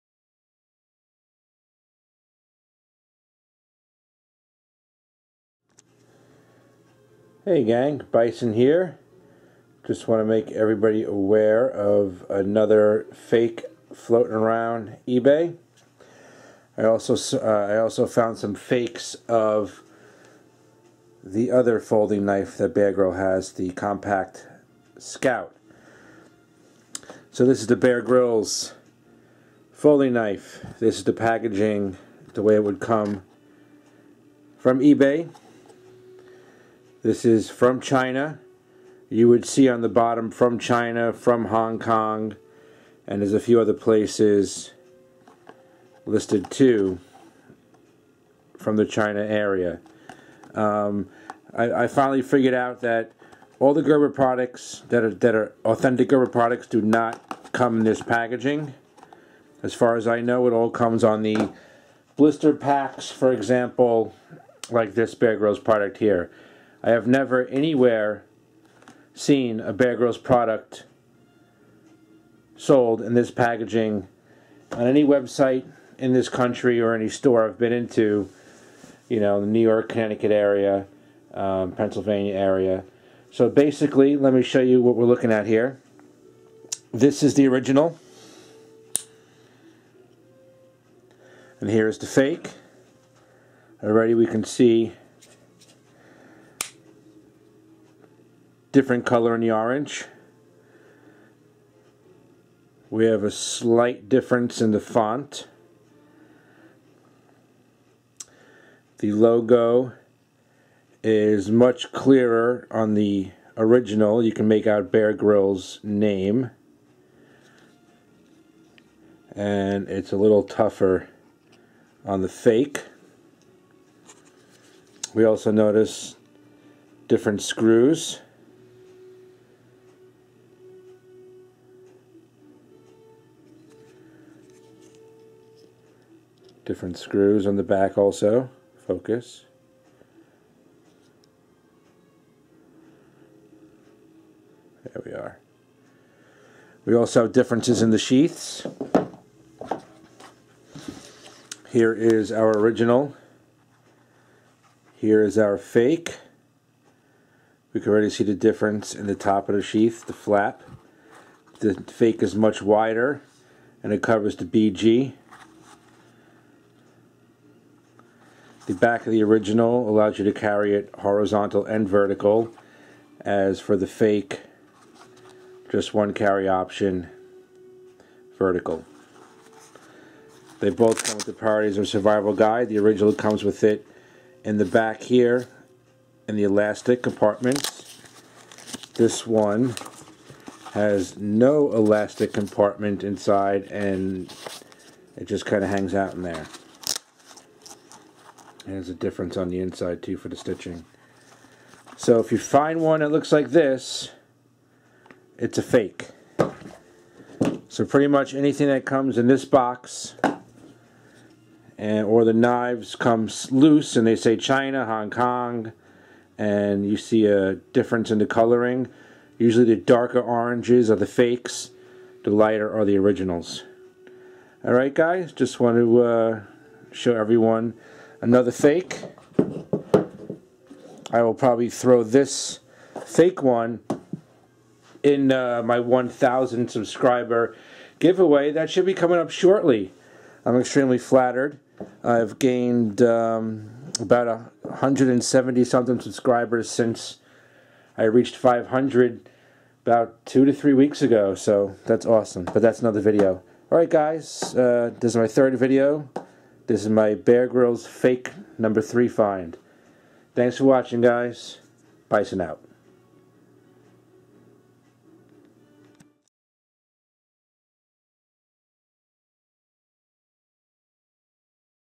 Another Bear Grylls Gerber Counterfeit Knife On Ebay
Another Fake on Ebay. The Bear Grylls Folding Sheath Knife.